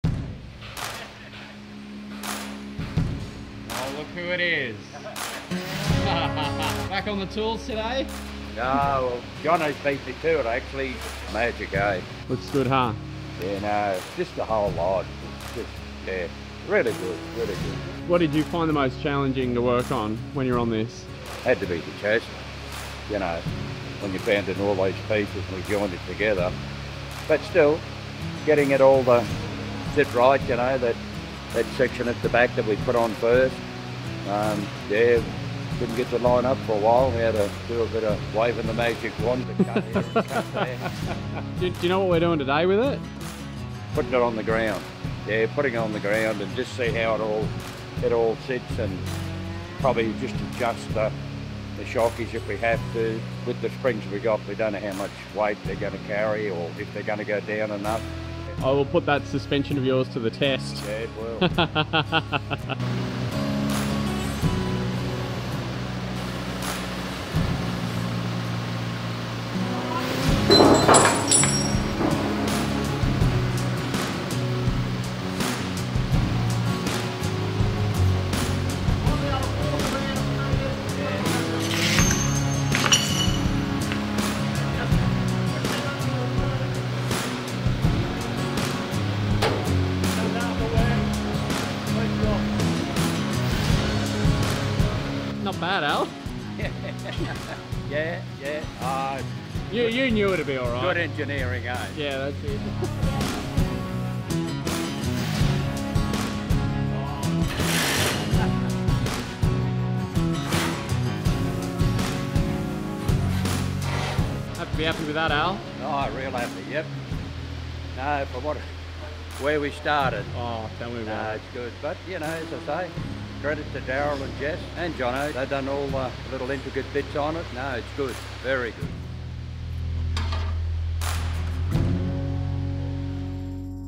Al says goodbye to the SdKfz 221 project (SUSPENSION TEST!)
Get your AusArmour Workshirt HERE!

Al drops in for the last time at the AusArmour Workshop to see how the suspension he made for the SdKfz 221 Armoured Car holds up!

Follow the progress of our workshop restorations every Workshop Wednesday! A must watch for students of history, engineering, mechanics and metalworking! 🧐🛠️